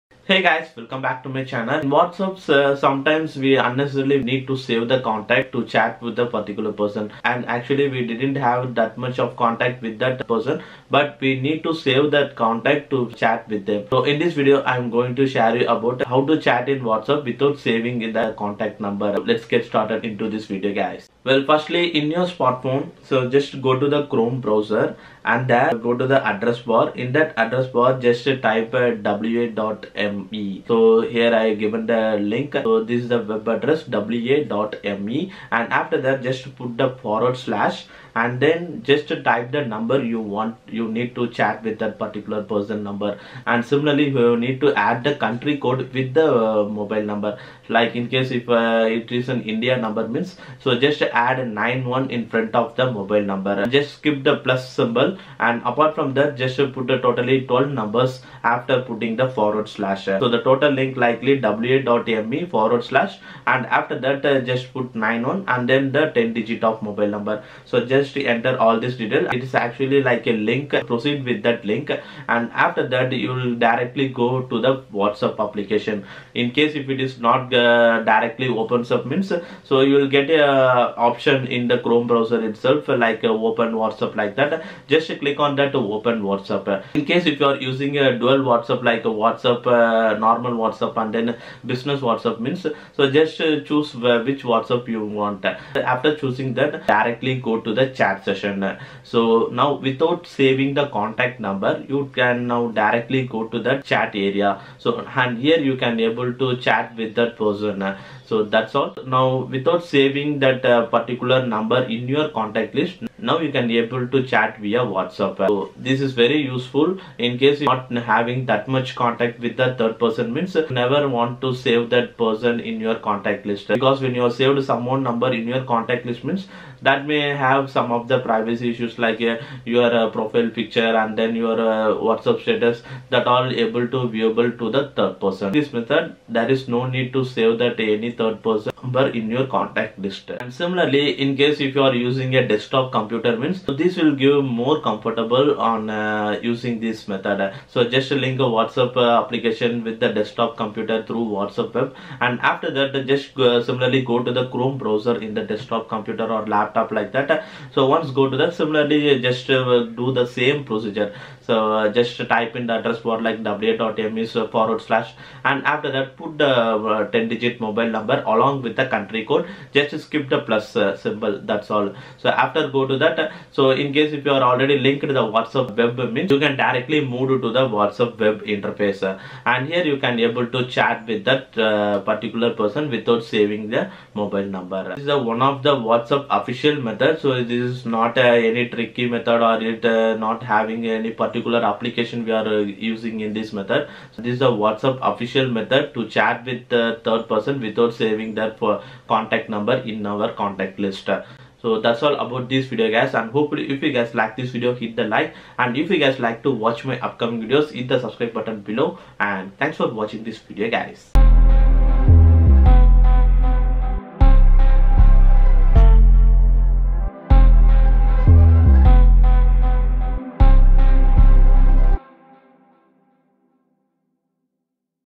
How to send WhatsApp messages without saving the phone number?
In this video, I'll show you how to send WhatsApp messages without saving phone numbers on your mobile.